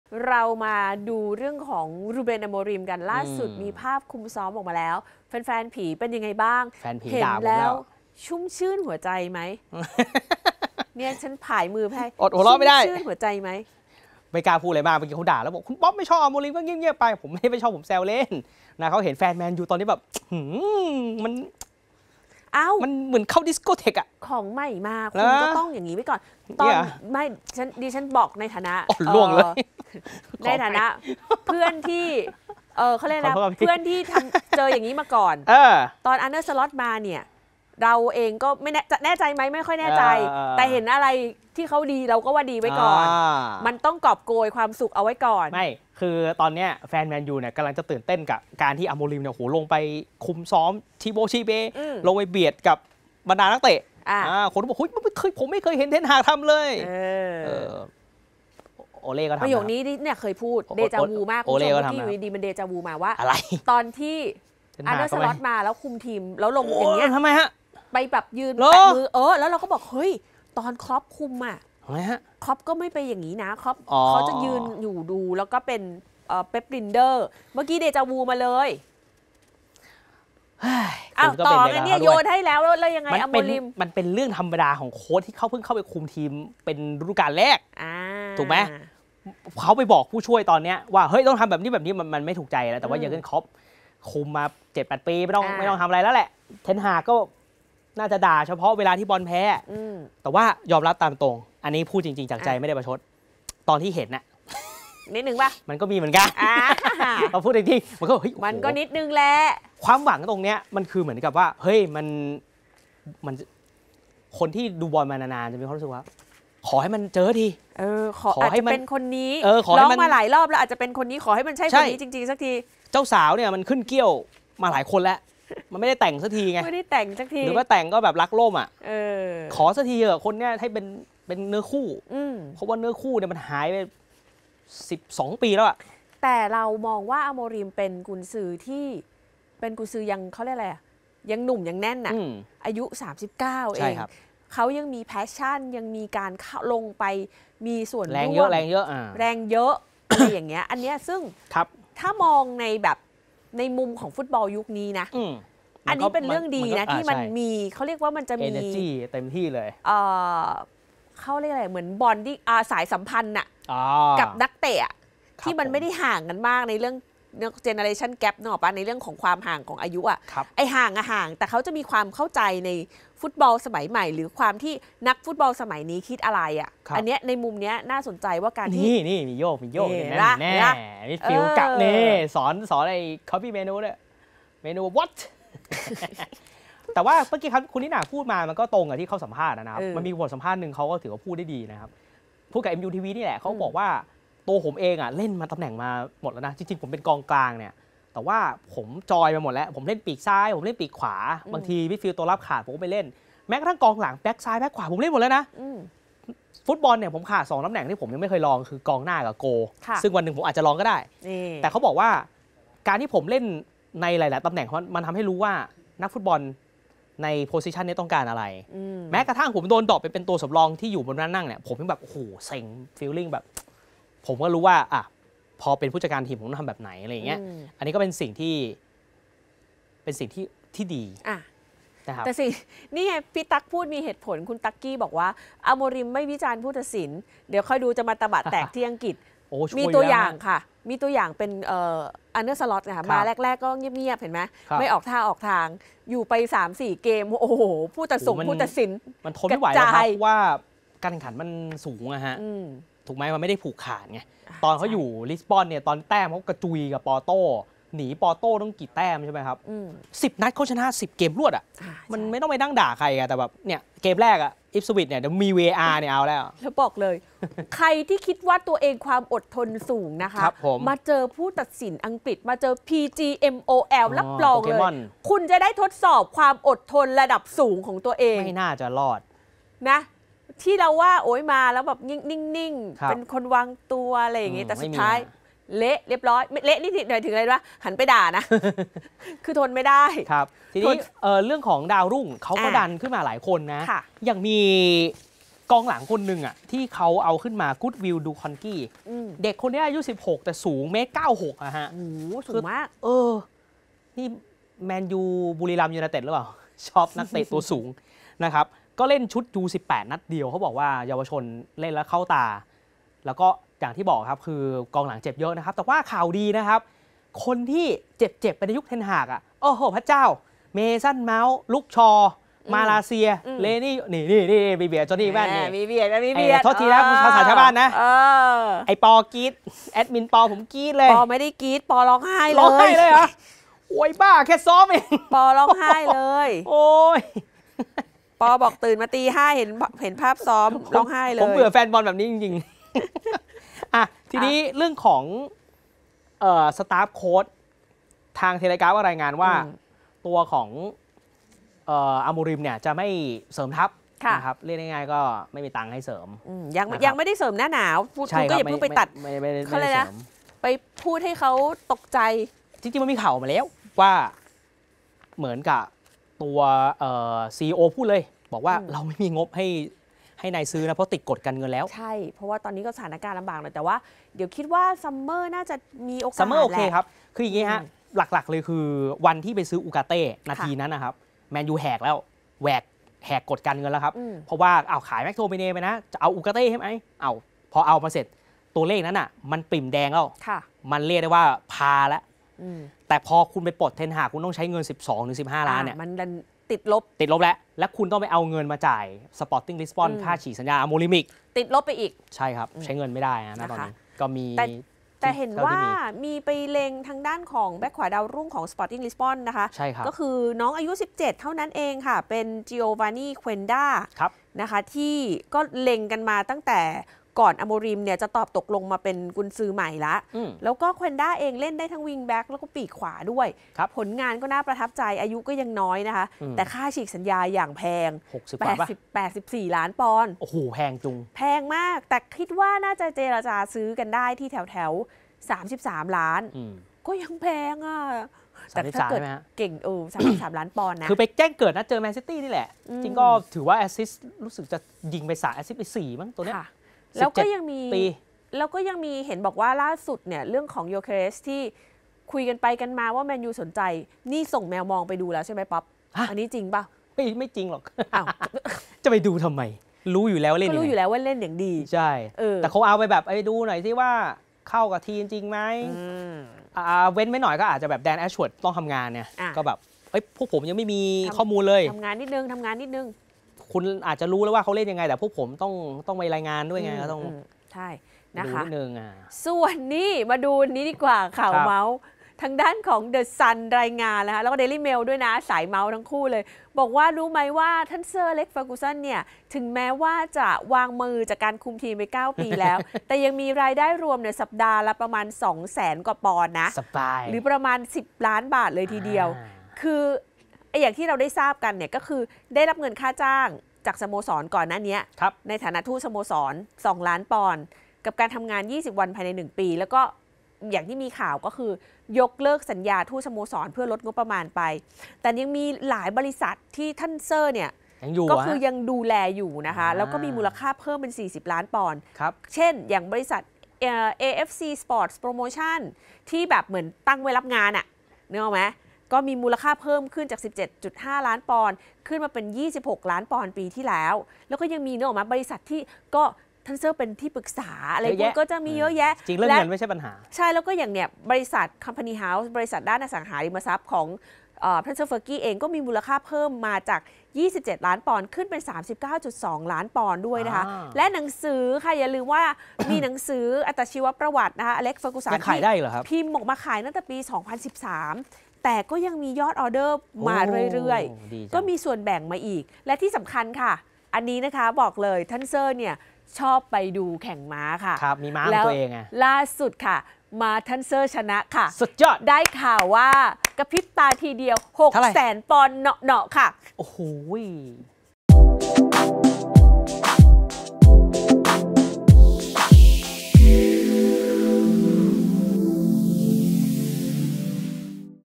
“อโมริม” คุม "แมนฯยู" ซ้อมวันแรกเรียกแข้งดาวรุ่งจอยเพียบ | SPORTS CORNER LIVE | 19 พ.ย. 67
รูเบน อโมริม ประเดิมคุม แมนเชสเตอร์ ยูไนเต็ด ฝึกซ้อมวันแรก หลังวีซ่าอนุมัติ พร้อมเรียกดาวรุ่งเข้ามาร่วมเซสชั่นหลายคน